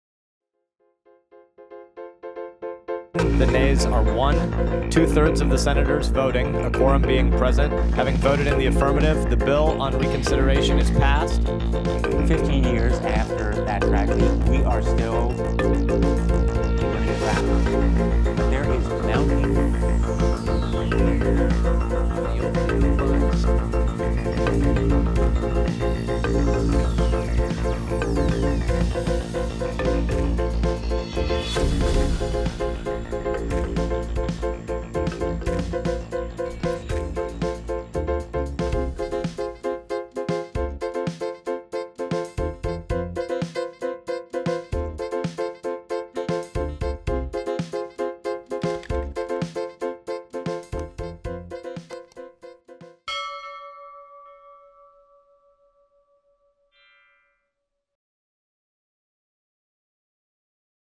مجلس الشيوخ الأمريكي يقرر رفع السرية عن وثائق حساسة تتعلق بهجمات ١١ سبتمبر | اورتز نيوز
القرار يمنح نحو عشرين ألف مدعي أميركي سلاحا لملاحقة السعودية بموجب قانون جاستا (العدالة ضد رعاة الإرهاب).